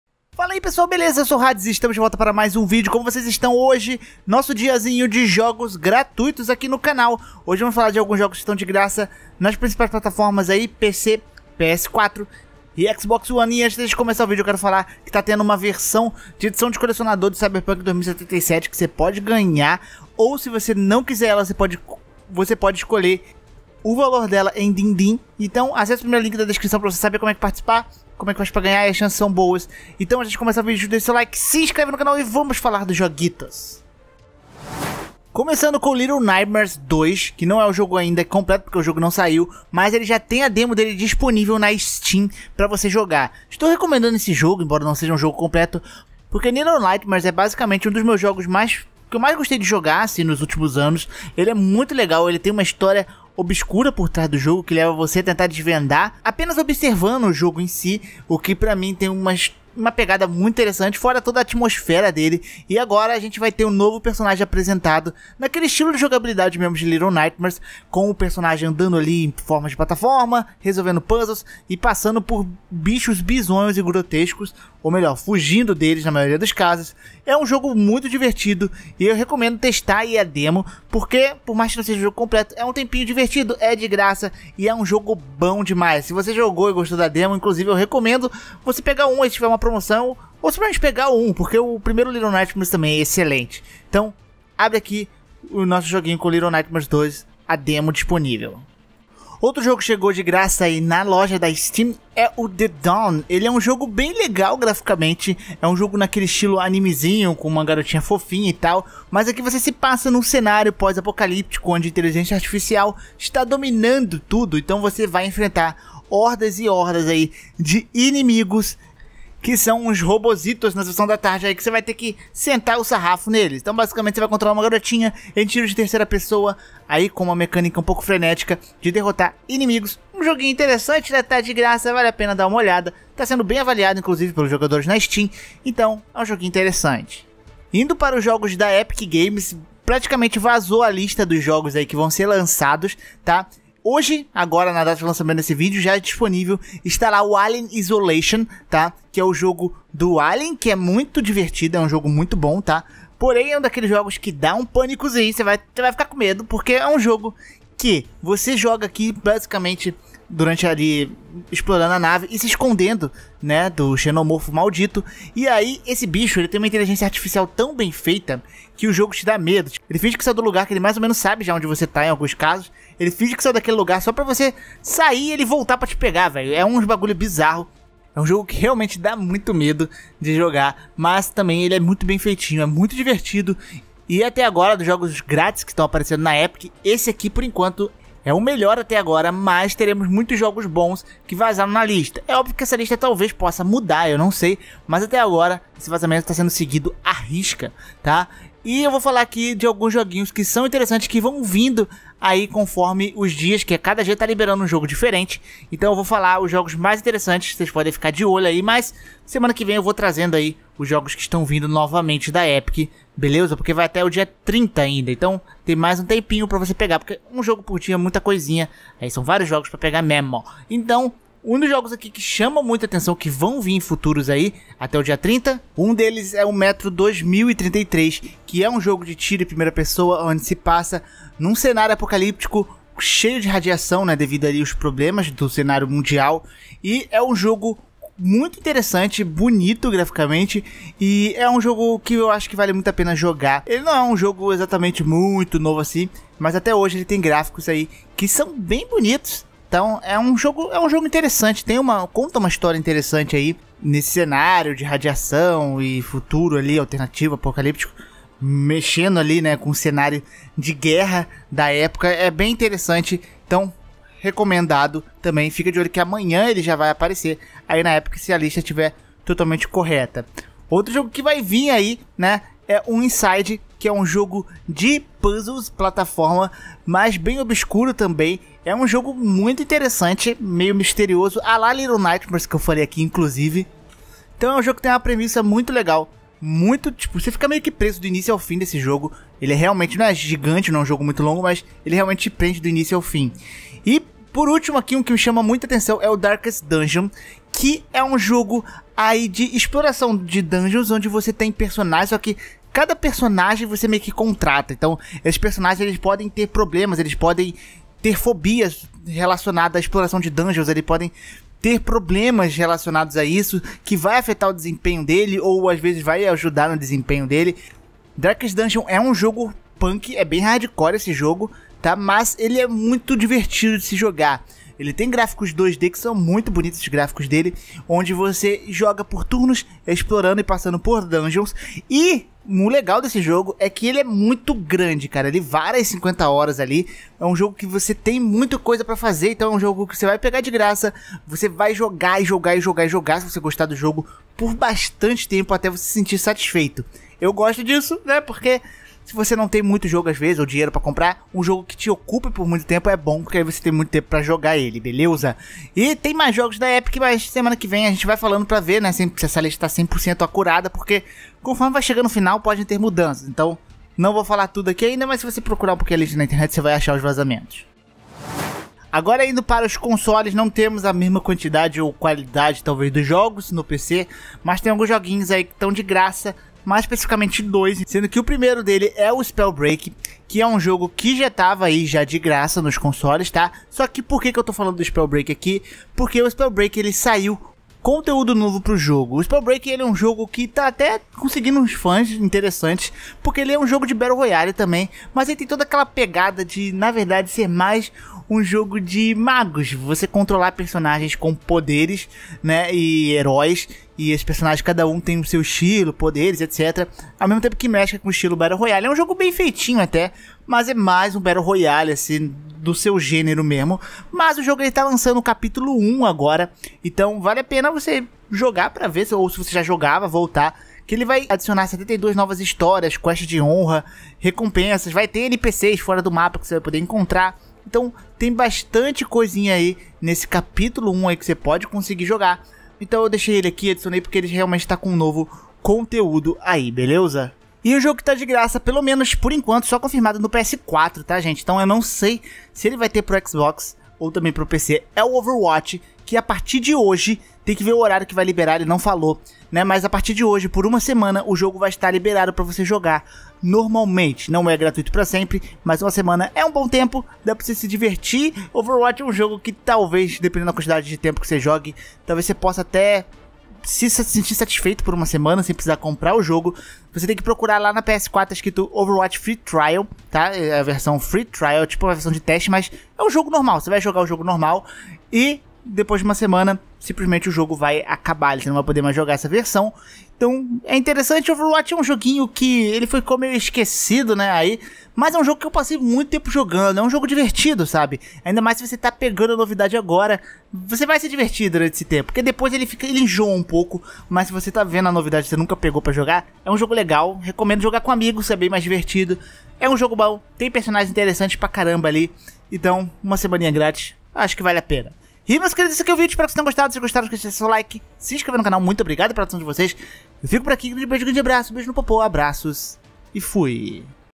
APROVEITA! Vários JOGOS pagos de GRAÇA! (PC, PS4, XONE)
➤ PSN: hadessama94

00:00 PC (Steam)
02:18 Epic Games
06:07 PS4 e XBOX